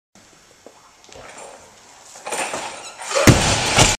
Donny Shankle Misses 170kg Snatch Over the back at California Strength Olympic Weightlifting
Donny Shankle Misses 170kg Snatch Over the back at California Strength Olympic Weightlifting coached by Glenn Pendlay